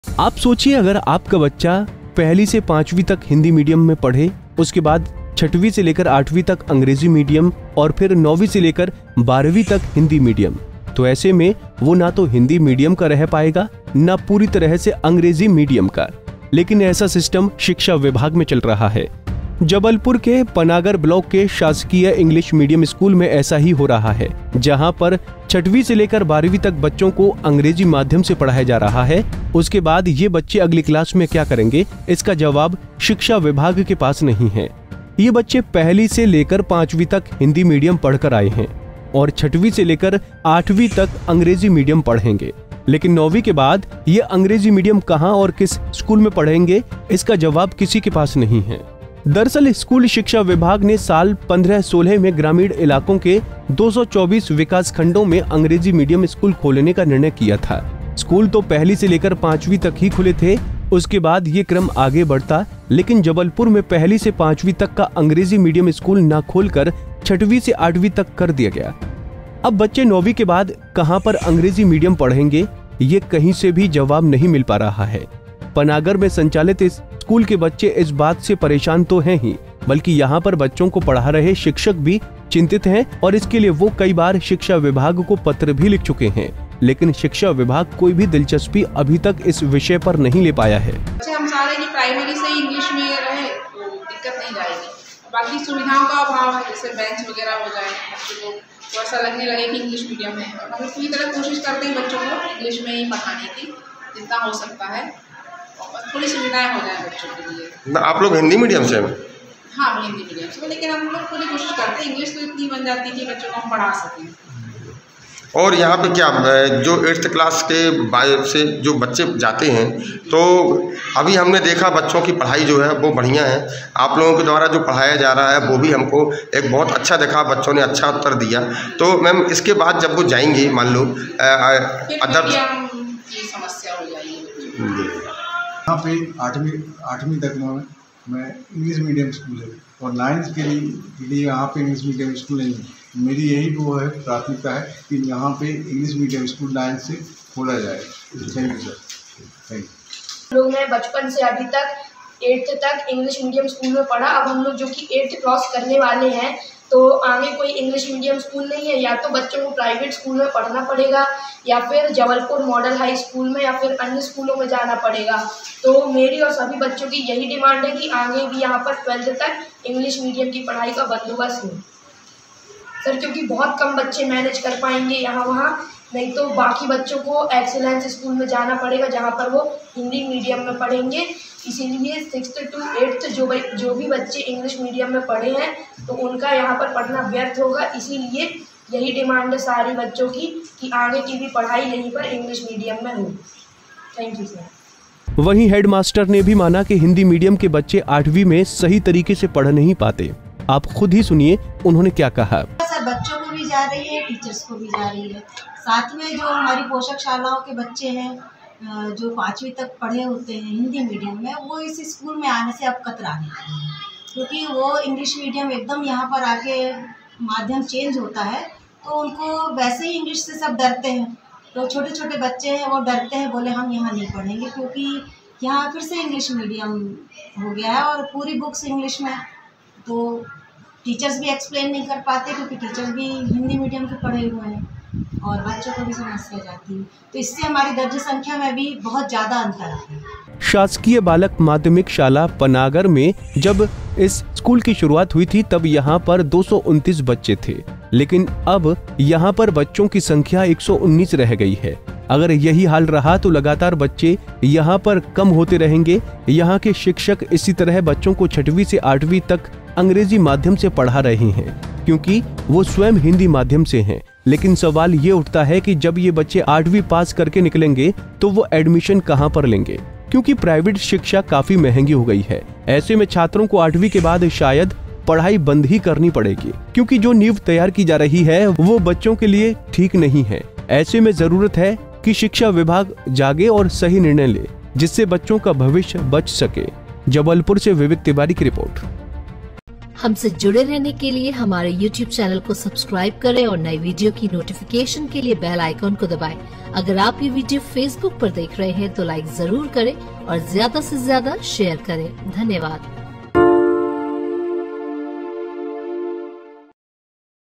5वीं तक पढ़ो हिंदी, फिर आगे अंग्रेजी मीडियम, MP के इस सरकारी स्कूल में खेला जा रहा बड़ा खेल
Also Watch ►